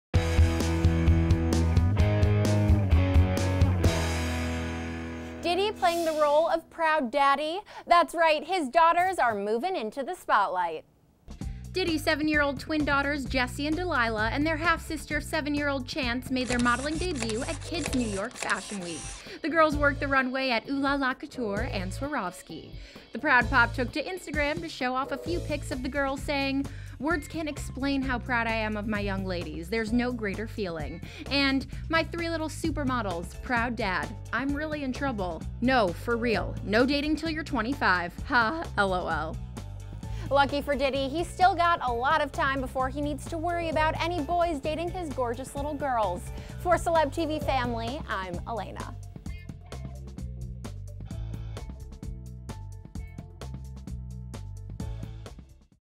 Diddy's Daughters Model at Kid's Fashion Week!
Diddy's 7-year-old twin daughters, Jessie and D'Lila, and their half sister, 7-year-old Chance, made their modeling debut at Kids New York Fashion Week. The girls worked the runway at Ooh! La La COuture and Swarovski. The proud pop took to Instagram to show off a few pics of the girls saying, "Words can't explain how proud I am of my young ladies! There's no greater feeling," and "My 3 little super models ProudDad...I'm really in trouble. No for real. No dating 'till you're 25!!!Haa lol." Lucky for Diddy these ladies still have a long time before it's time to starting dating!
category: Entertainment, gossip, hollywood celebrity, Music